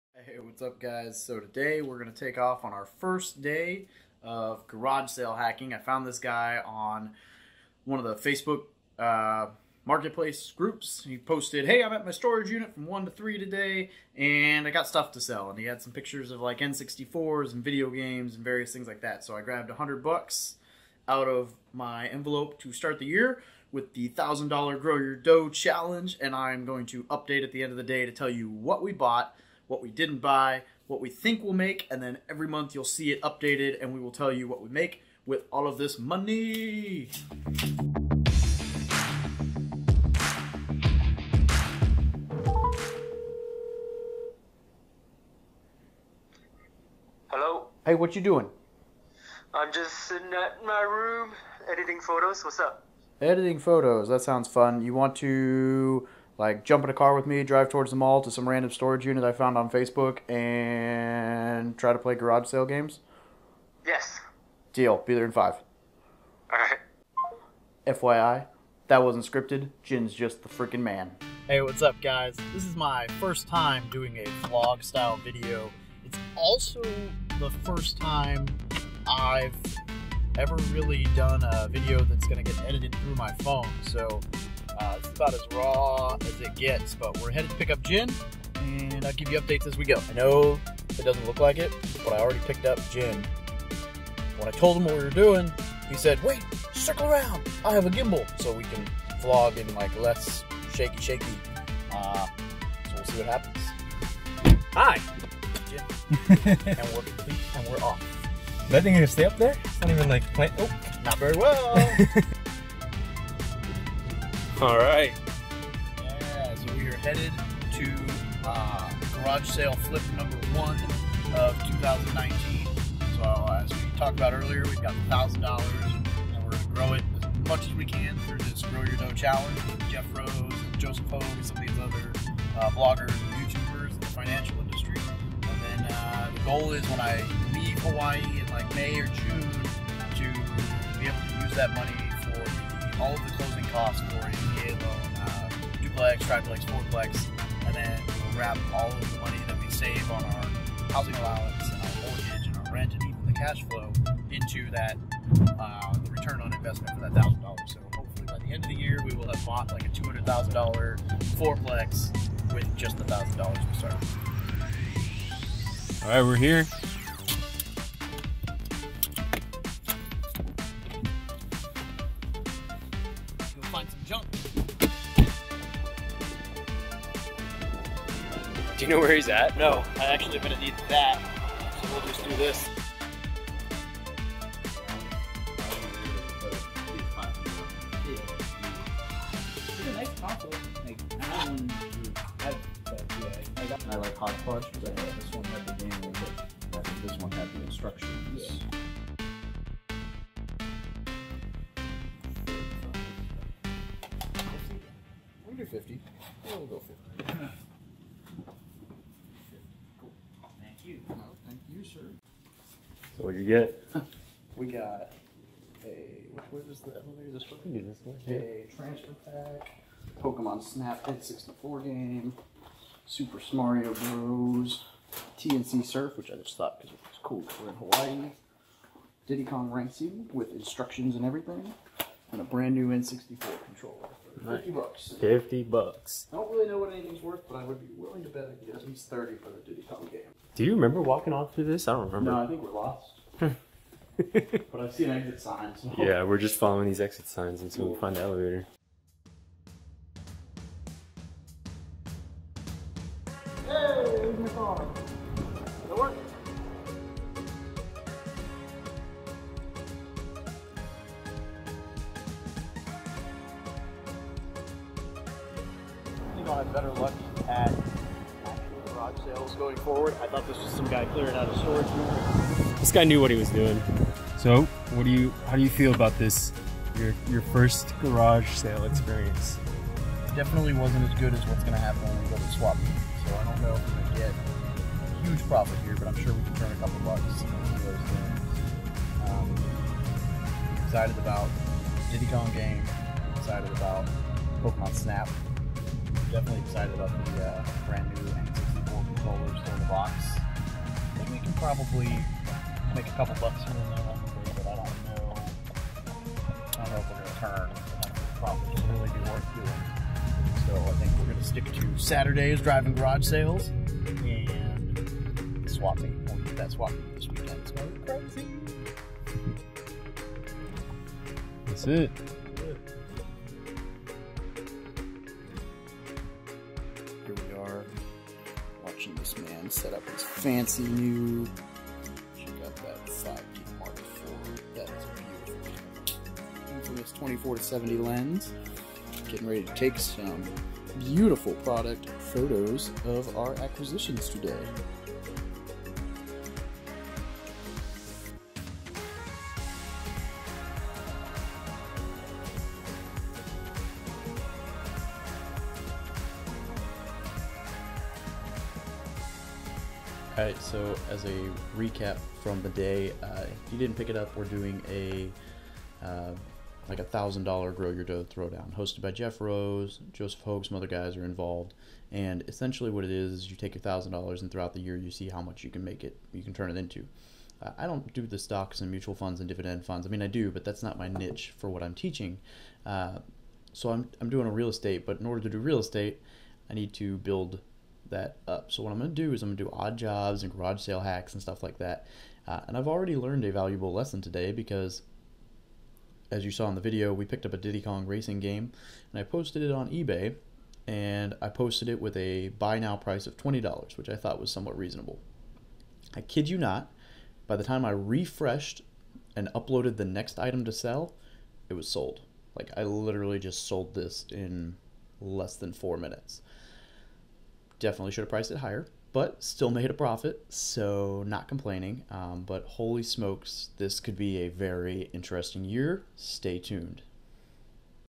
growyourdough The Grow Your Dough Throwdown Begins!
The 2019 Grow Your Dough Throwdown consists of bloggers in the financial community investing $1,000 on January 1st, in order to see how much wealth they can accumulate in the year!
I will be participating in this challenge utilizing the strategy known as arbitrage. I will be buying garage sale items for cheap, and then flipping them on craigslist, Ebay, at flea markets Etc.
This summer I will be moving back to southern California. Once I make this move, I will be using the money I've accumulated to purchase a house-hack! I plan to cover all costs associated with this purchase with the Grow Your Dough funds!
At this time of this video being uploaded I have already bought (6) items for $50, then turned around and sold (2) of those items for $50…leaving the remaining (4) items to be purely profit!
Channels Involved in this Challenge